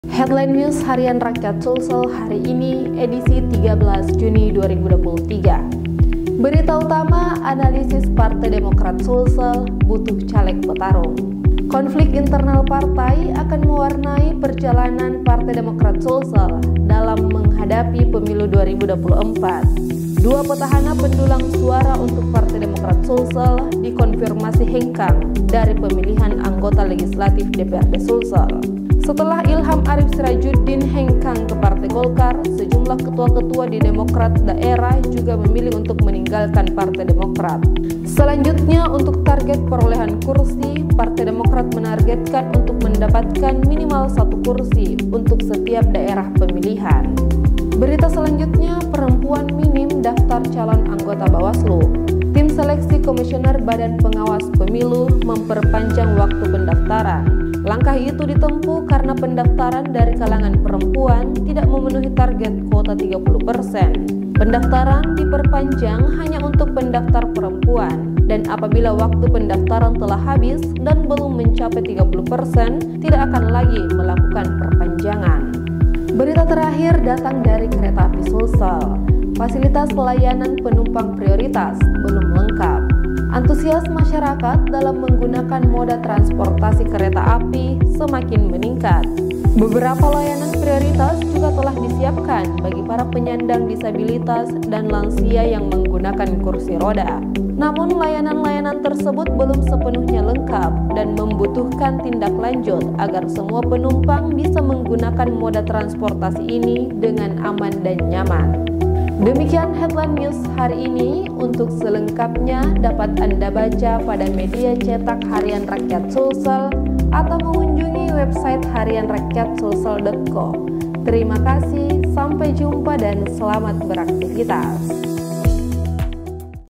Headline News Harian Rakyat Sulsel Selasa 13 Juni 2023 | Partai Demokrat Sulsel
1. (Partai Demokrat Sulsel) Butuh Caleg Petarung
2. Perempuan Minim Daftar Calon Anggota Bawaslu
3. Fasilitas Layanan Penumpang Prioritas Belum Lengkap